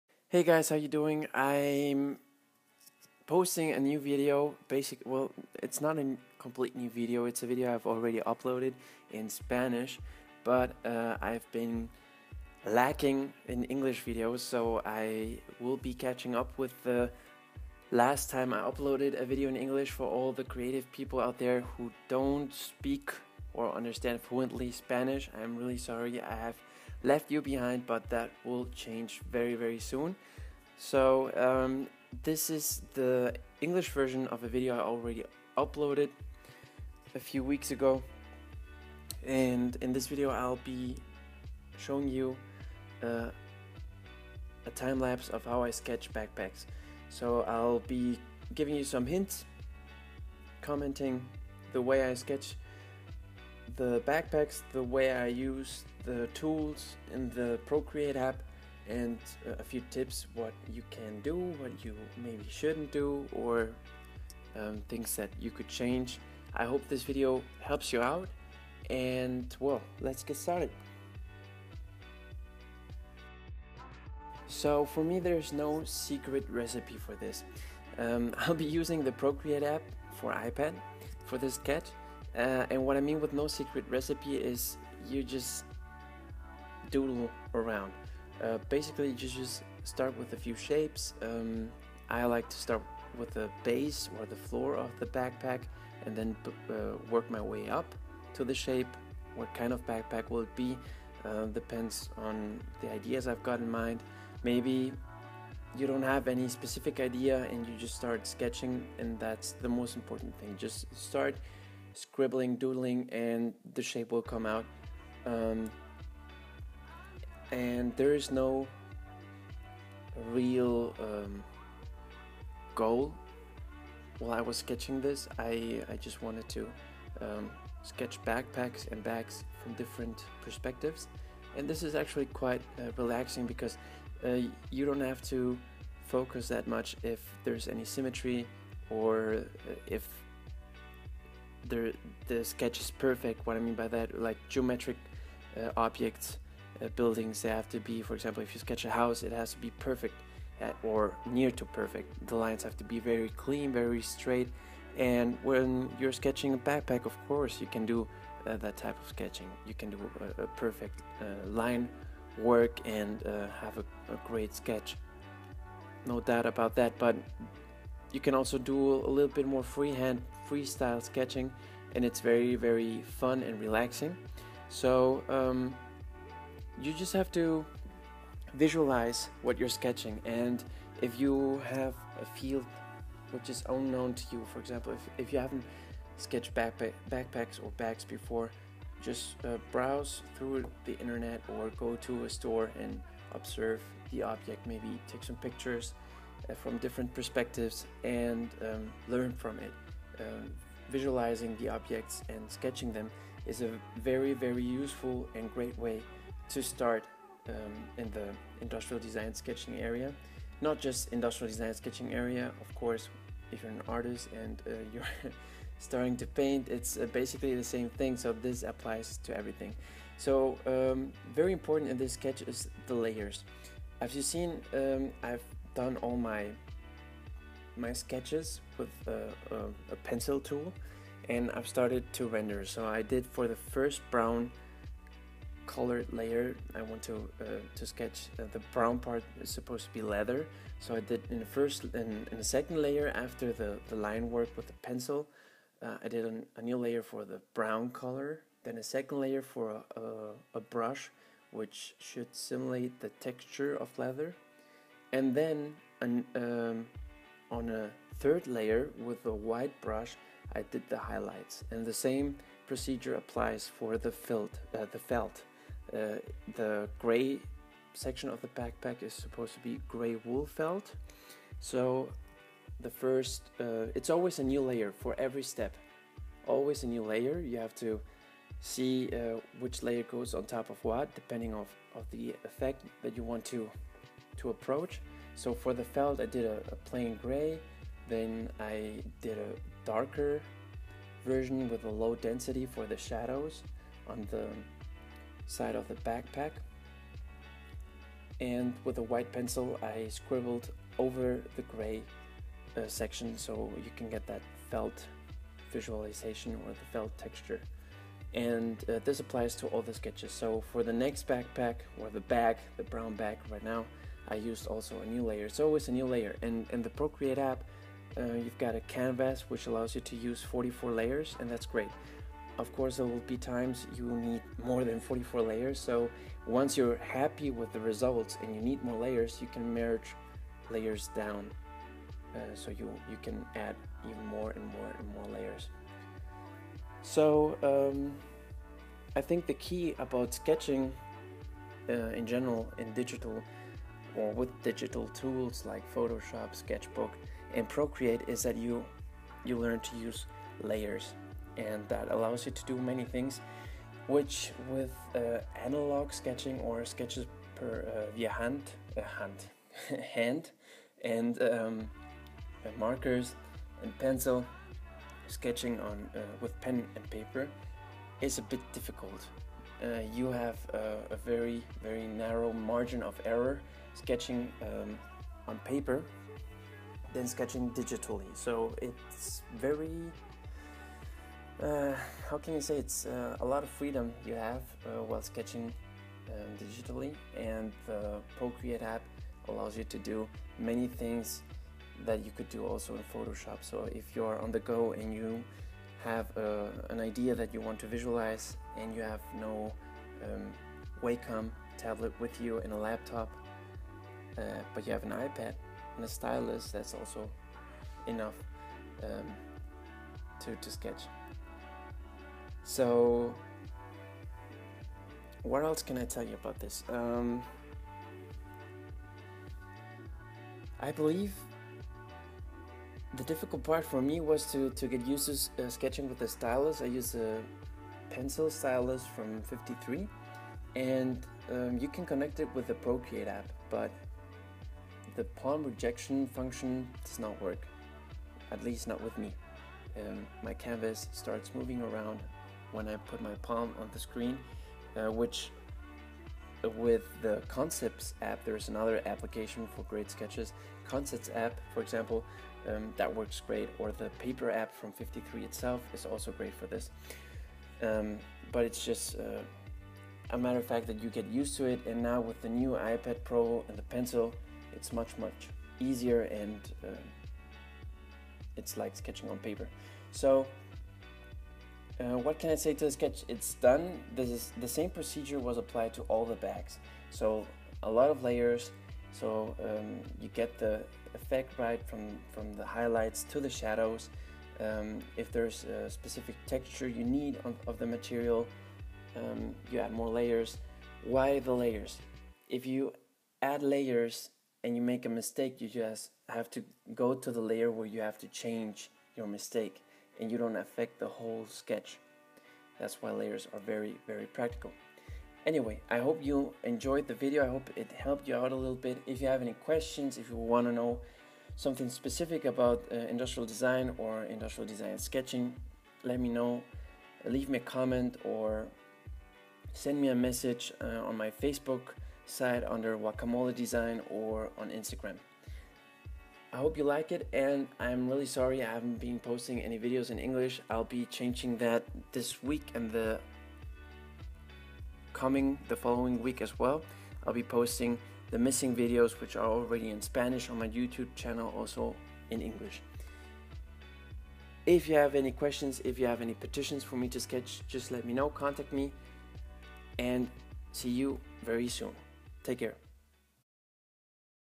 How to sketch backpacks on Procreate
Hey guys!!
I'm really sorry for the delay with my videos in english!! I promise I'm working hard to keep them up to date!
Here's a quick tutorial about how you can sketch backpacks using the Procreate app! In this video I give you a few hints and tips which will hopefully help you while sketching!